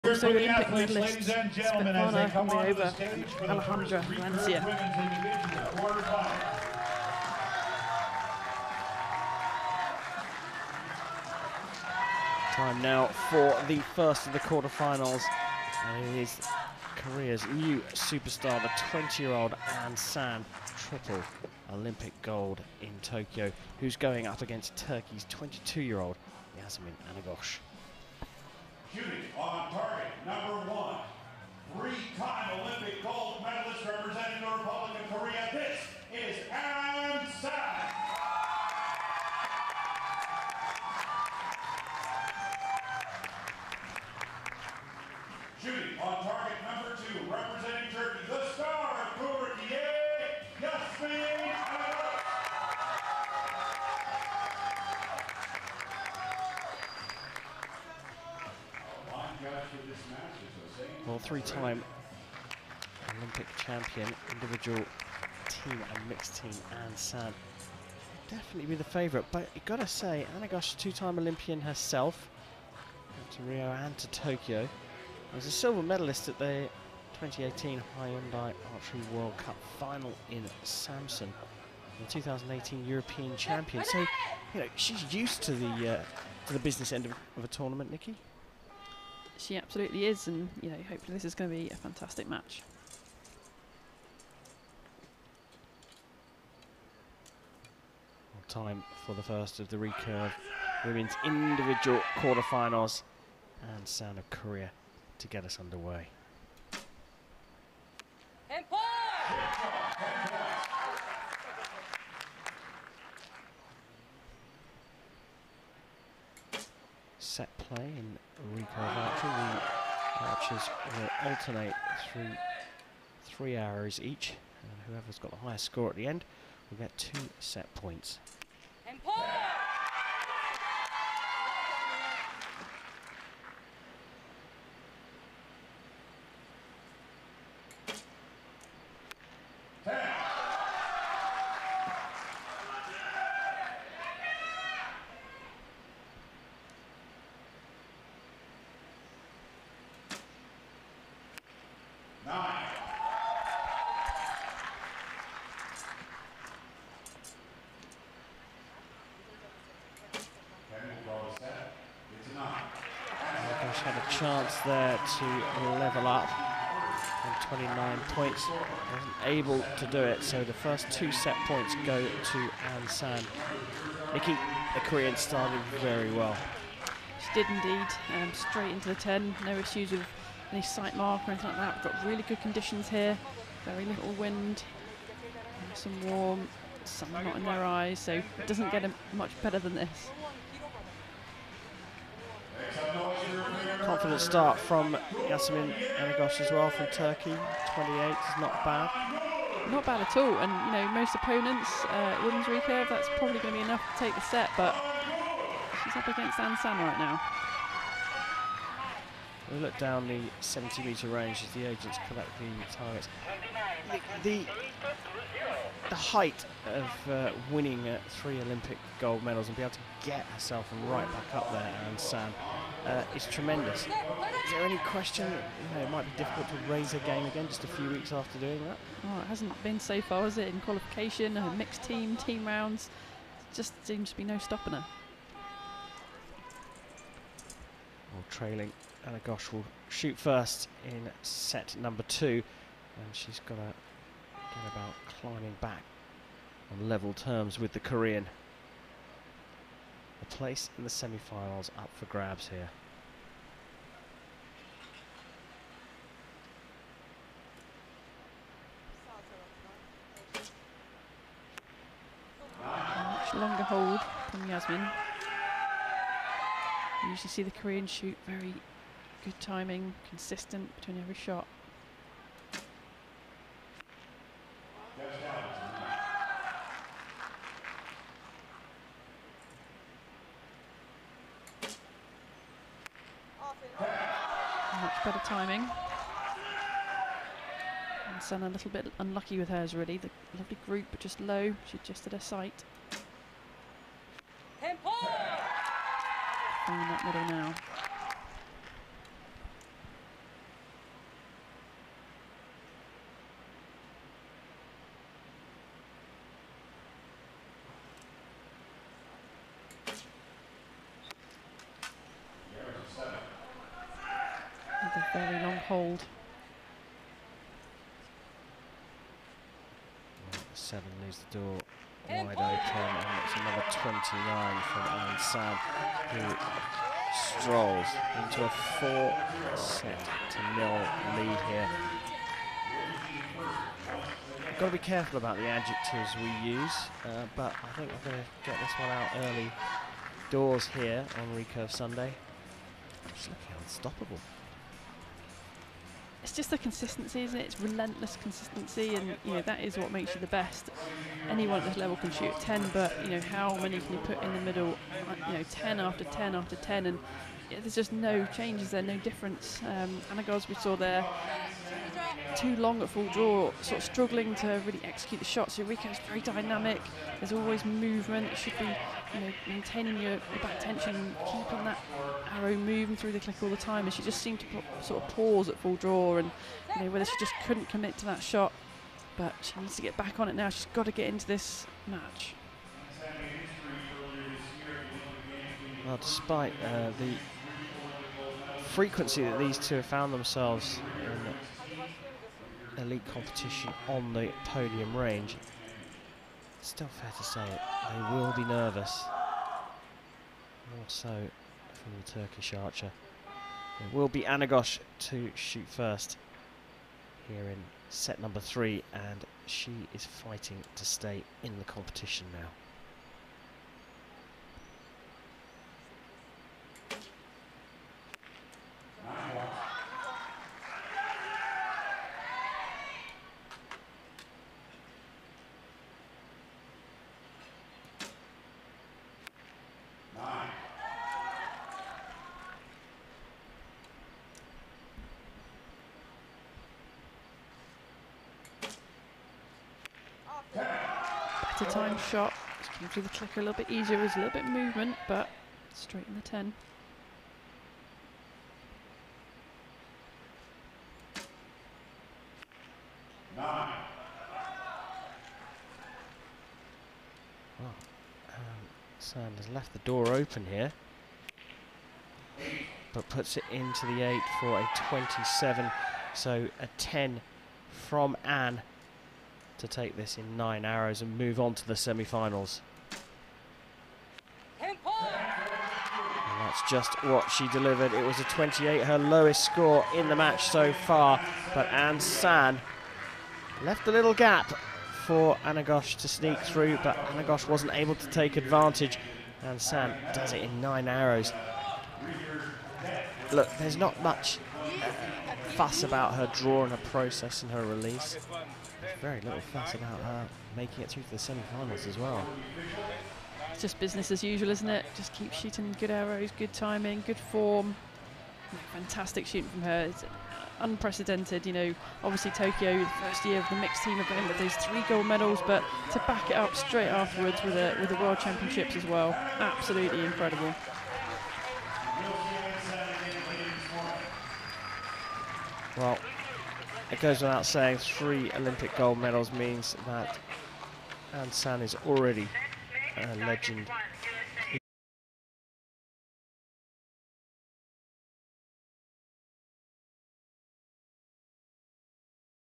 An San v Yasemin Anagoz – recurve women quarterfinal | Yankton 2021 World Archery Championships
Recurve women's individual quarterfinal match between An San of Korea and Yasemin Anagoz of Turkey at the 2021 Hyundai World Archery Championships in Yankton, USA. Recurve archers shoot at a target 70 metres away, 122cm in diameter and the central 10-ring measures just 12.2 centimetres in diameter (about the size of a DVD).